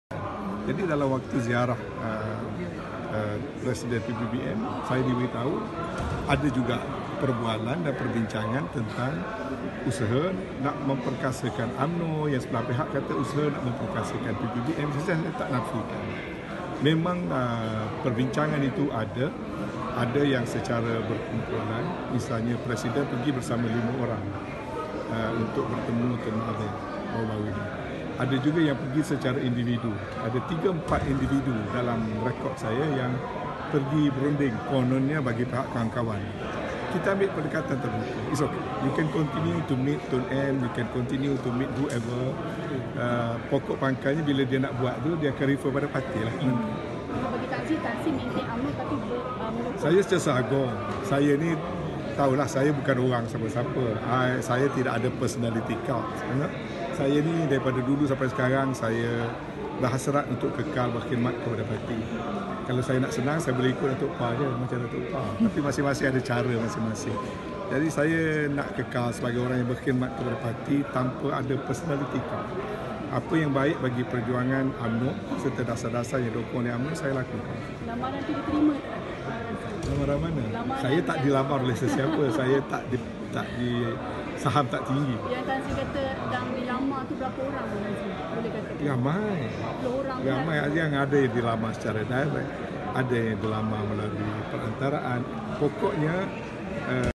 扎希偕党员见敦马 安努亚拒多加猜测
巫统总秘书丹斯里安努亚慕沙说，他身为党总秘书，以开放的态度看待巫统党员随同党主席或个人身分，去会见土着团结党主席敦马哈迪和总裁丹斯里慕尤丁，商讨强化巫统和土团党的事宜。（本报徐健华摄）